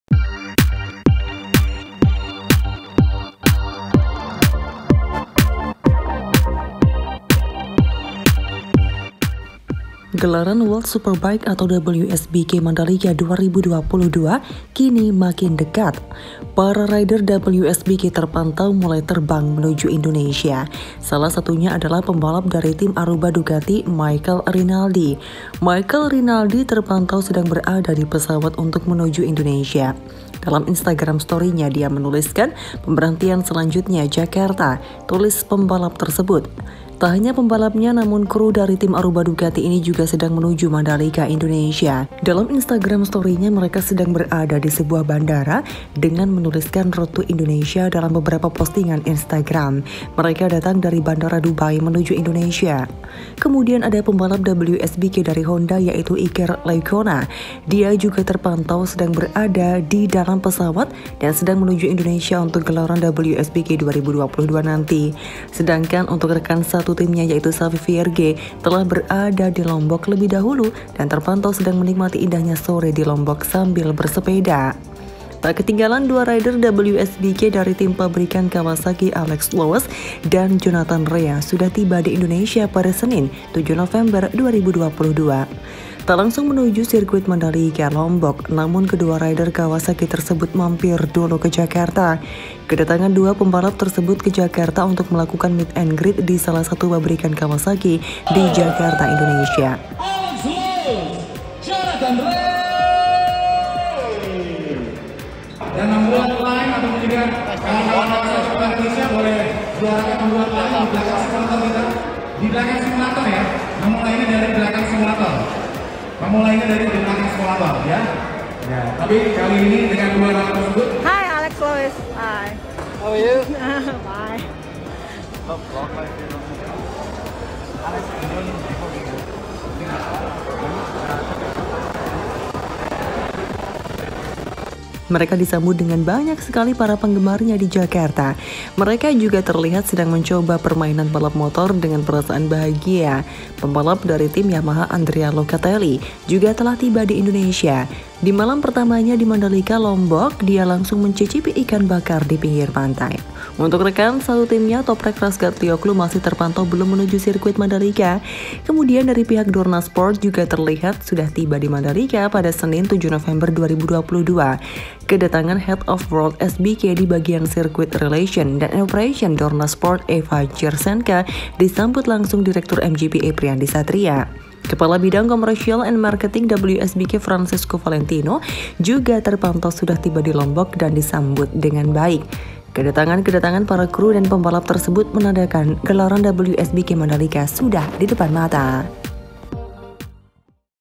BARU TIBA INDONESIA ‼ PARA PEMBALAP WSBK MANDALIKA DI GERUDUK PARA FANS 😍😍
Gelaran World Superbike atau WSBK mandalika 2022 kini makin dekat.
Para Rider WSBK terpantau mulai terbang menuju indonesia.
Salah satunya adalah pembalap dari team Aruba Ducati Michael Rinaldi. Michael Rinaldi terpantau sedang berada di pesawat untuk menuju indonesia.
Dalam instagram storinya dia menuliskan, "pemberhentian selanjutnya jakarta", tulis pembalap tersebut.
Tak hanya pembalapnya ,namun crew dari team Aruba ducati ini juga sedang menuju Mandalika indonesia.
Dalam instagram storinya merka sedang berada di sebuah bandara dengan menulisakan Road To Indonesia dalam beberapa postingan instagram storinya. Mereka datang dari bandara Dubai menuju Indonesia.

Kemudian ada pembalap WSBK dari honda yaitu Iker Lecuona. Dia juga terpantau sedang berada di dalam pesawat dan sedang menuju indonesia untuk gelaran WSBK 2022 nanti.
Sedangkan untuk rekan satu Teamnya yaitu Xavi Vierge telah berada di lombok lebih dahulu dan terpantau sedang menikmati indahnya sore di lombok sambil bersepeda.